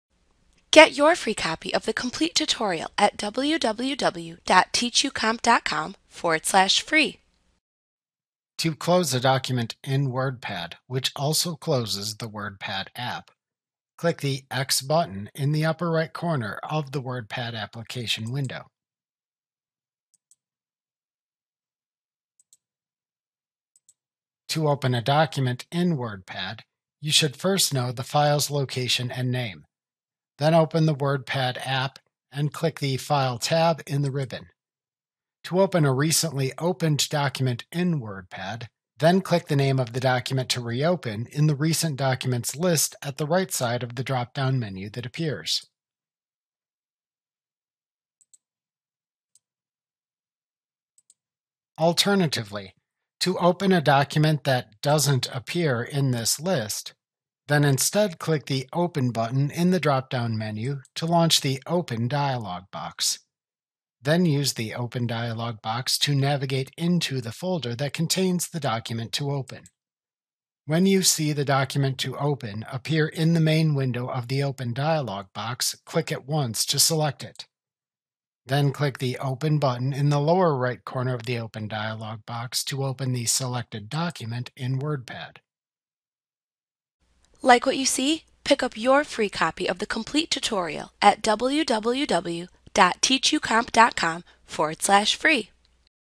Learn How to Close and Open a Document in WordPad in Windows 11: A Training Tutorial
Learn how to close and open a document in WordPad in Windows 11 A clip from Mastering Windows Made Easy.